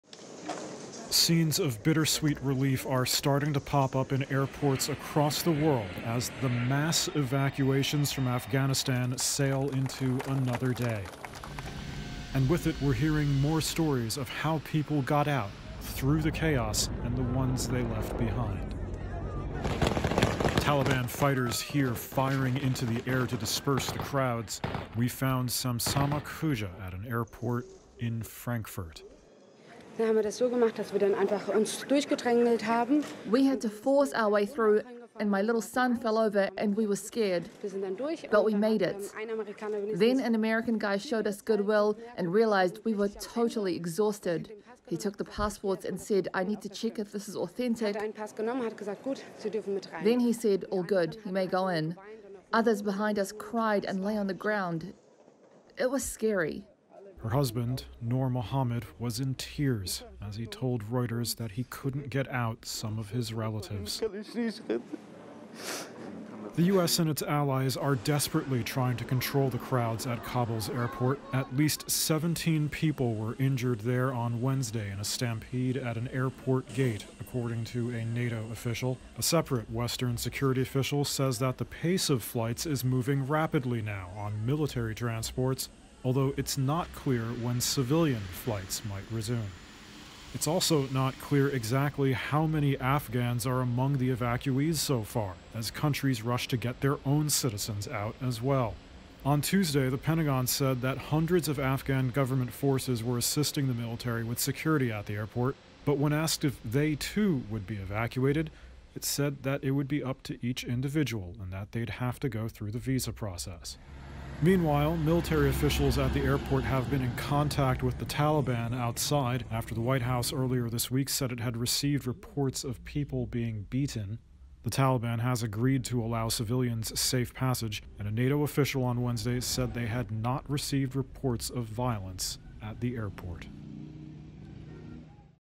Afghan evacuations gather pace, but crowds remain
As evacuations gathered pace in Afghanistan, people shared their stories about how they got out through the chaos in Kabul airport. Most of them said they feared for the lives of loved ones left behind.